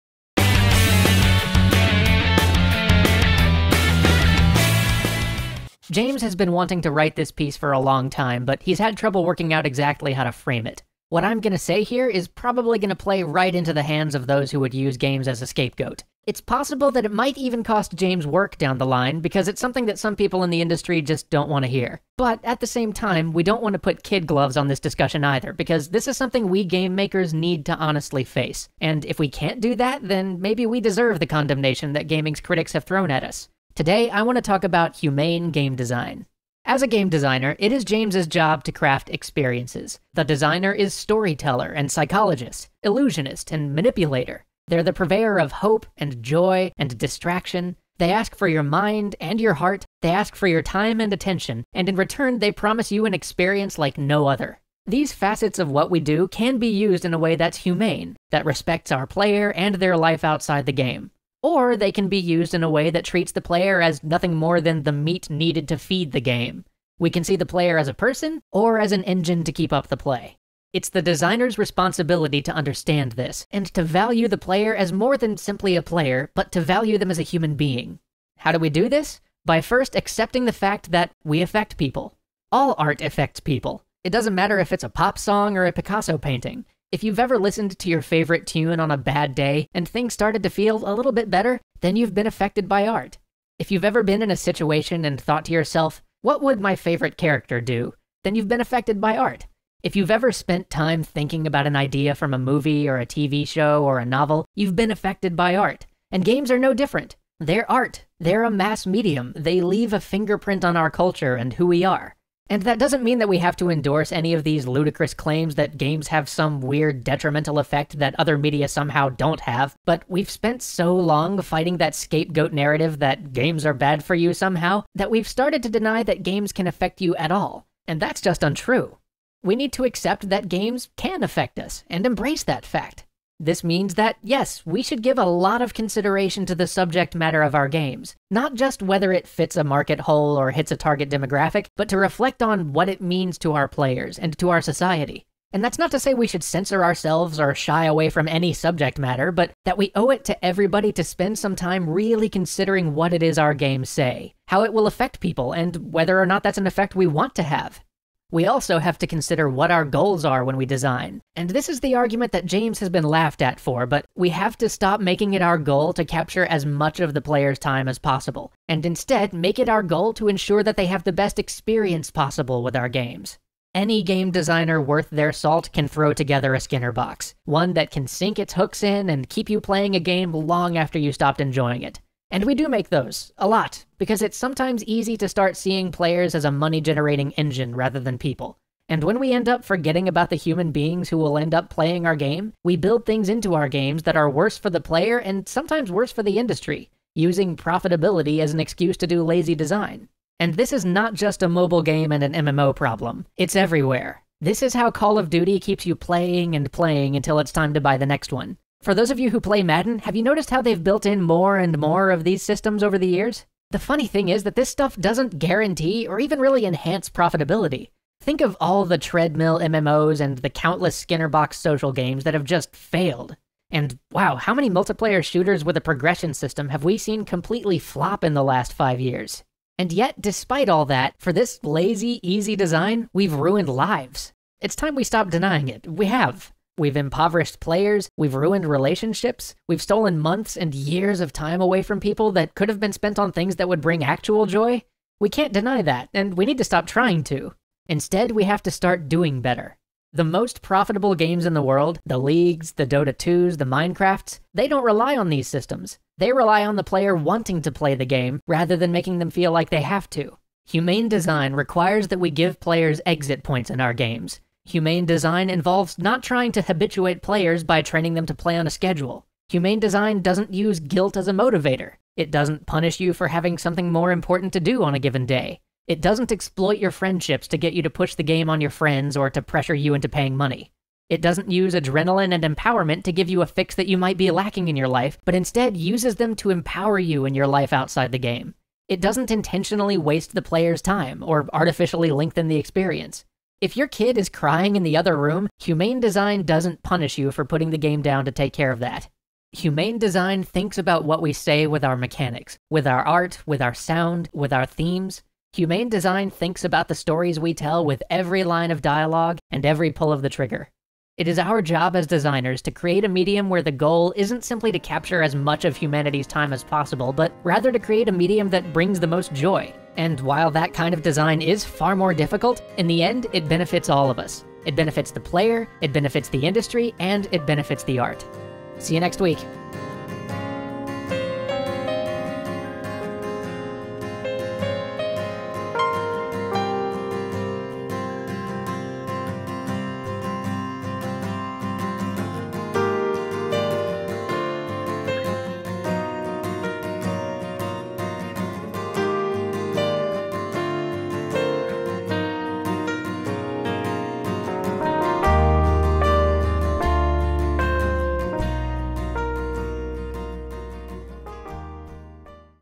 Humane Design - Games Must Be Good to Their Players - Extra Credits
Games do not dictate our behavior, but they do affect us. They can affect us in positive ways, by letting us share experiences with other people or giving us a refuge from the troubles of our daily lives. They can also affect us in negative ways, however, by building Skinner Box traps that teach us to play for rewards, not for fun, and by monetizing our basic human interests instead of helping us explore them.